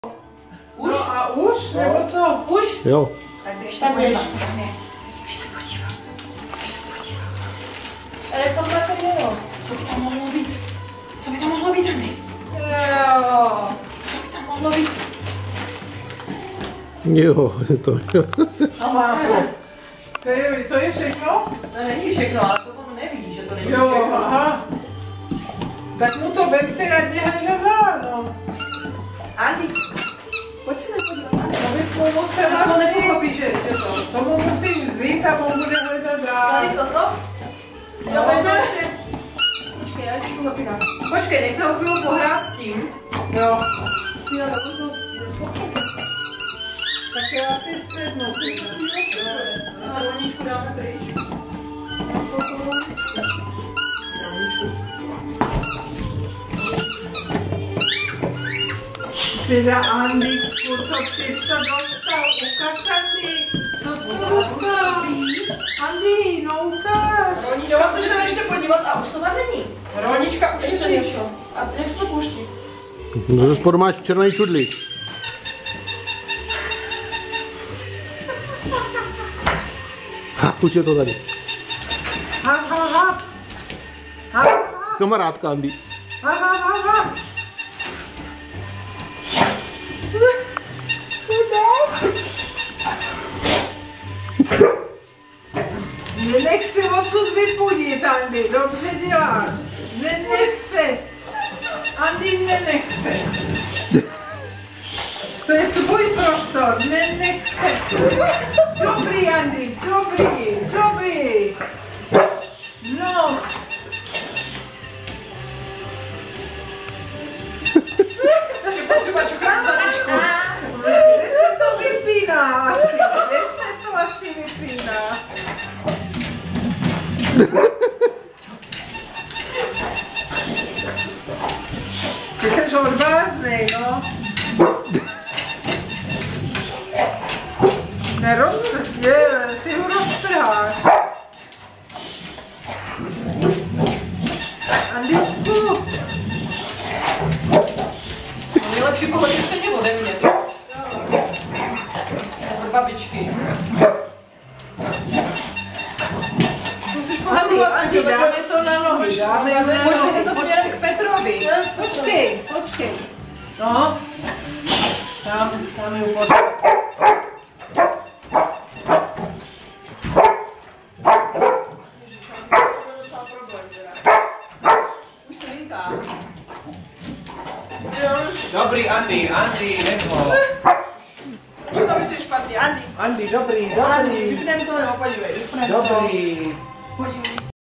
Andy pod vánočním stromečkem . Vánoce 2011
Andýsek si přebírá dárky pod stromečkem.